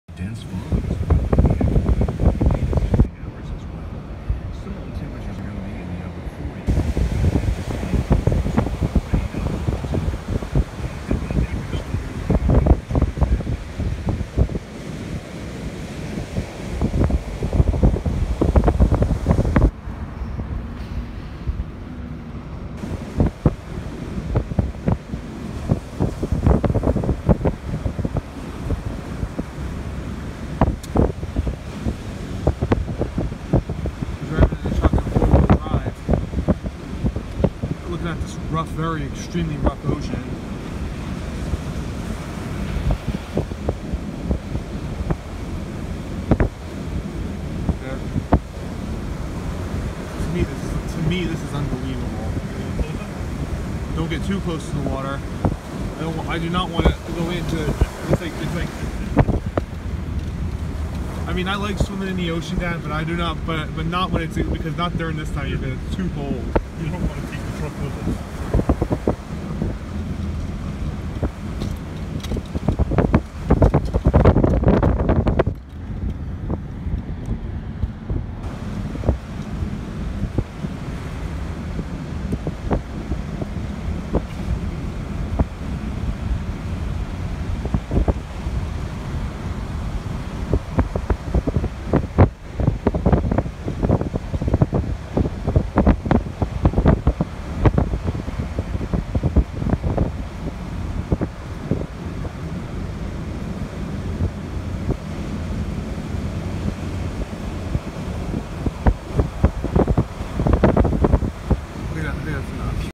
Driving on the beach 🏖
My dad and I were riding on the beach with a 4 wheel drive!